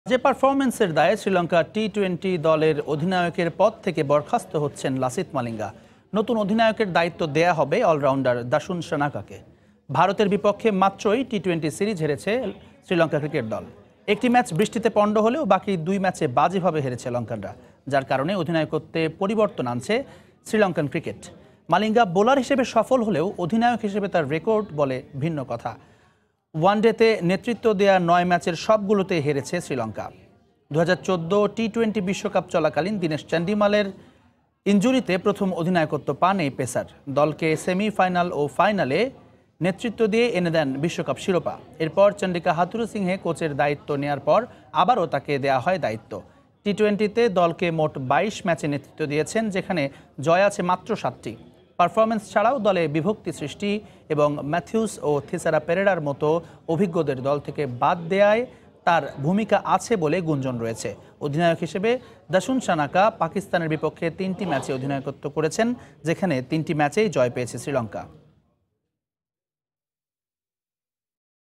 টি-২০ অধিনায়কত্ব থেকে বরখাস্ত লাসিথ মালিঙ্গা ! | Lasith Malinga | Sports News | Somoy TV
দলের বাজে পারফরম্যান্সের দায়ে শ্রীলঙ্কা টি-২০ দলের অধিনায়কের পদ থেকে বরখাস্ত হচ্ছেন লাসিথ মালিঙ্গা। নতুন অধিনায়কের দায়িত্ব দেয়া হবে অলরাউন্ডার দাসুন শানাকাকে।